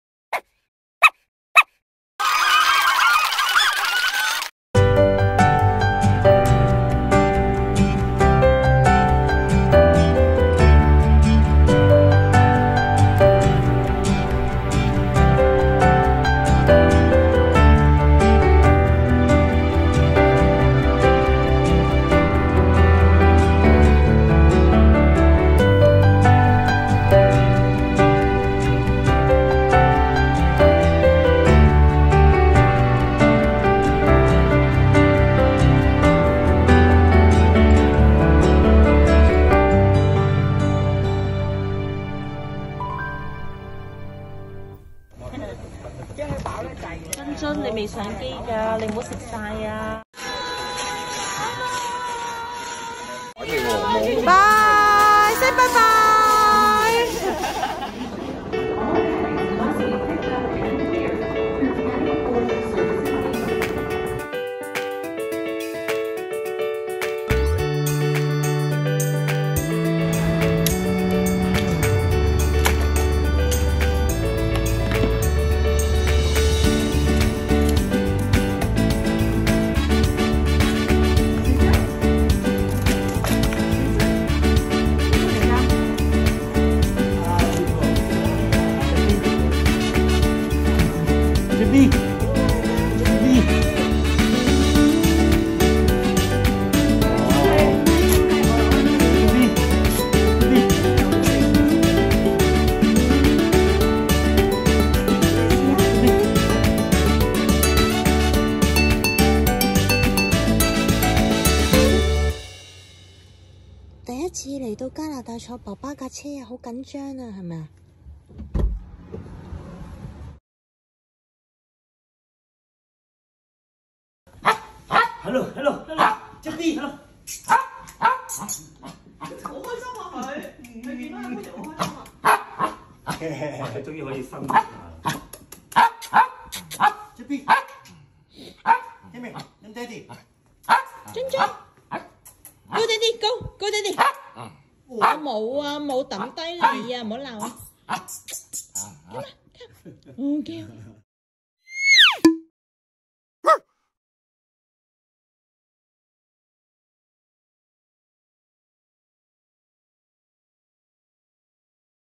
小樽婆婆千里尋爸記 Travelling with my 14-year-old dog to reunion with daddy in Canada | BE Pawtique
14歲小樽婆婆勇闖溫哥華之旅. 經歷16小時飛越太平洋與爸爸團聚
A BIG BIG project for her and for me.
Bringing my 14-year-old princess flying from Hong Kong to Canada to reunion with daddy. She is such a brave girl being so calm and peaceful during the entire journey.